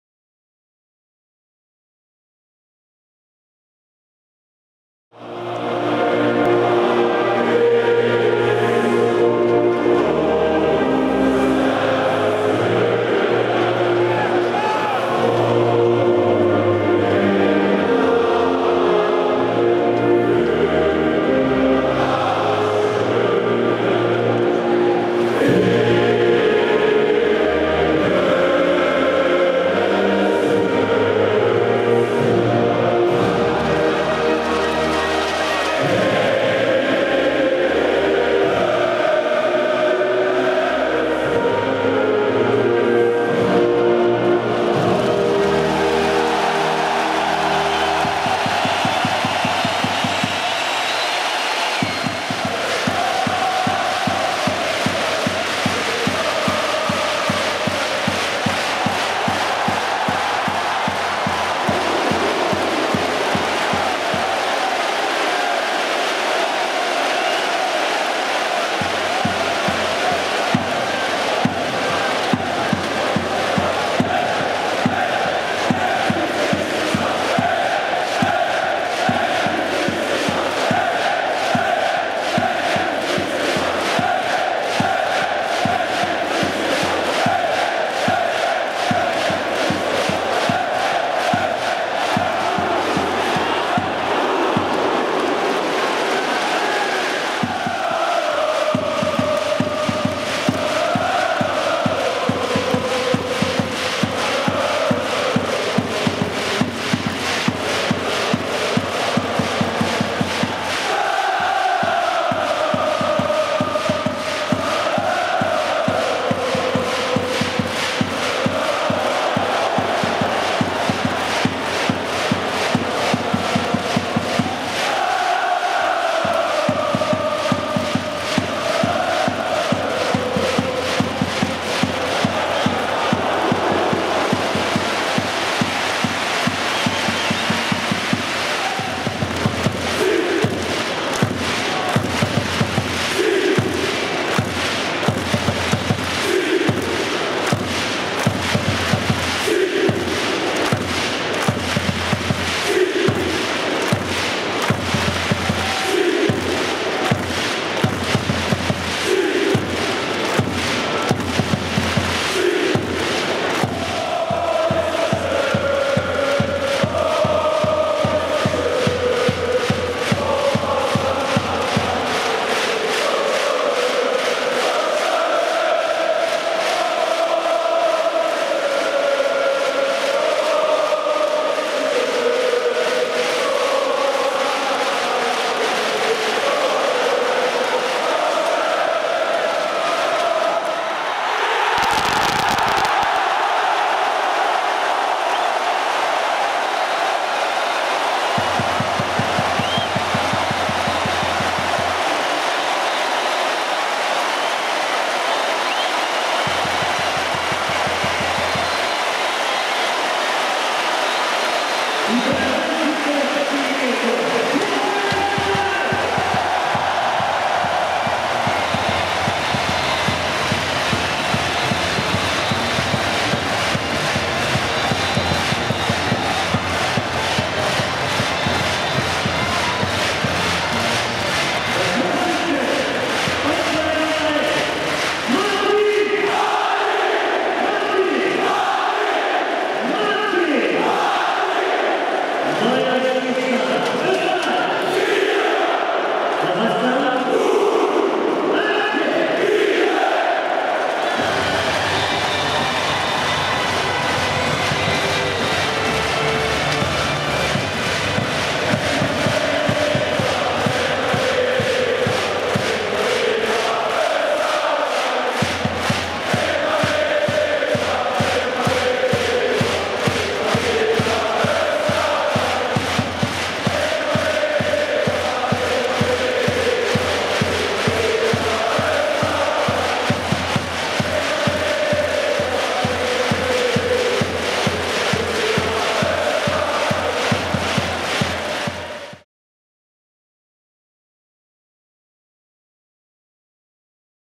Österreich - Kasachstan // Austria - Kazakhstan 4:0, 16.10.2012 (Hurricanes)
Dies ist der Zusammenschnitt der Hurricanes des WM-Qualifikationsspieles zwischen Österreich und Kasachstan (4:0) am 16. Oktober 2012 im Wiener Ernst Happel Stadion.